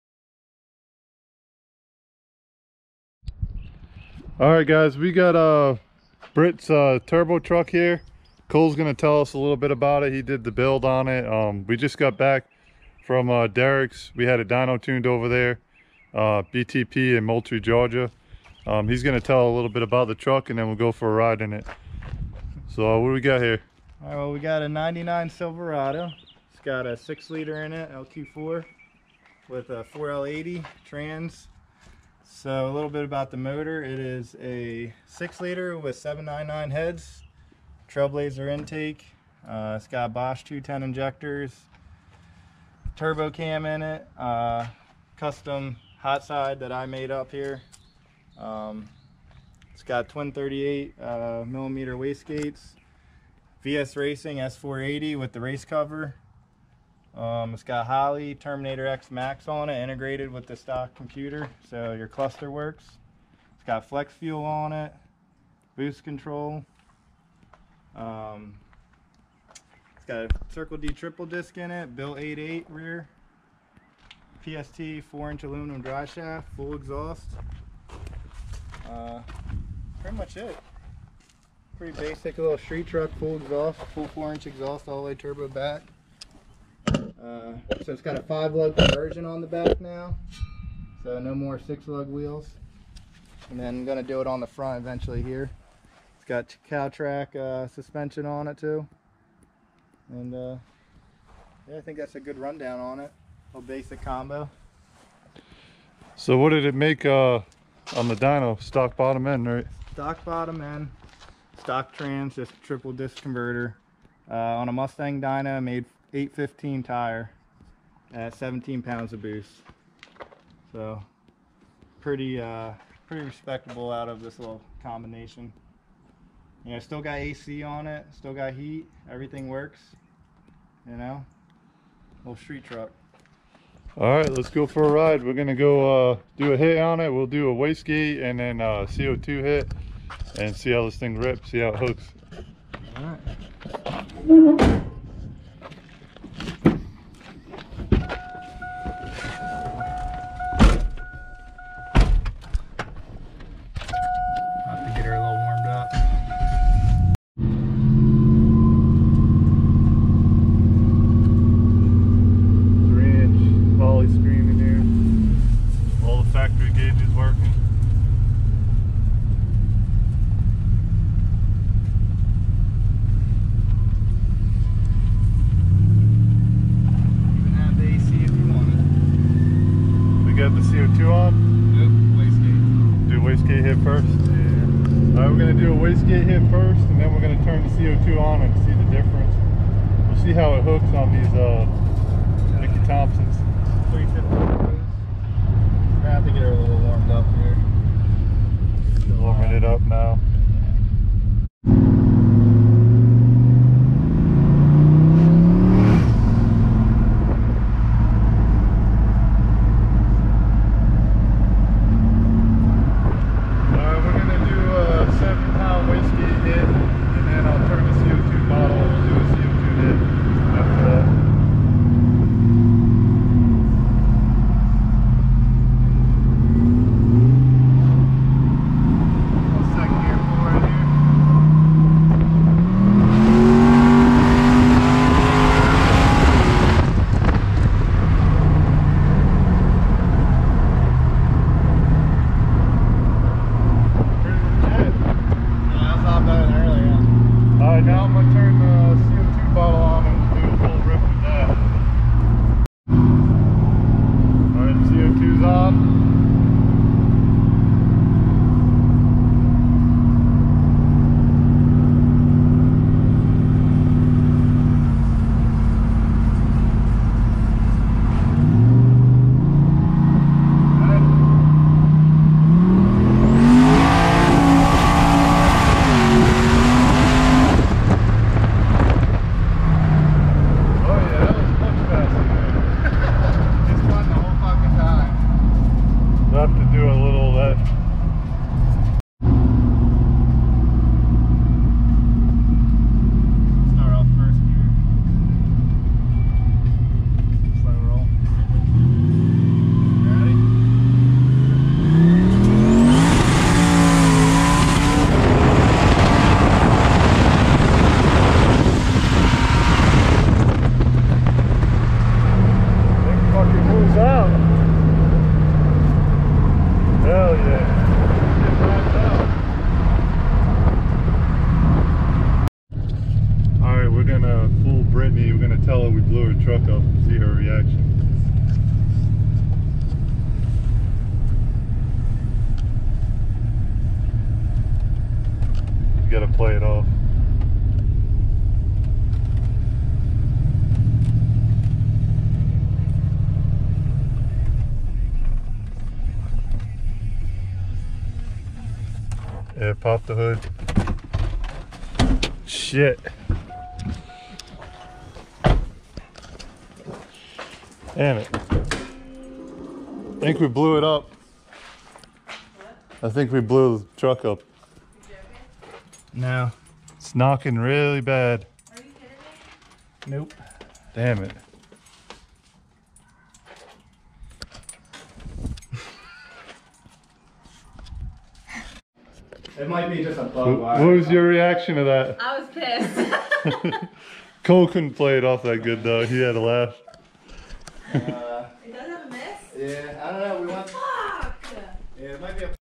800+ Stock bottom end Turbo Silverado Ride Along
6.0 stock bottom end, 799 heads, Vs racing 80mm turbo, 4l80 with triple disk Circle D converter, E85, 8.8 rear 410 gears, BTP tune. Truck rips... Stay tune for more builds and videos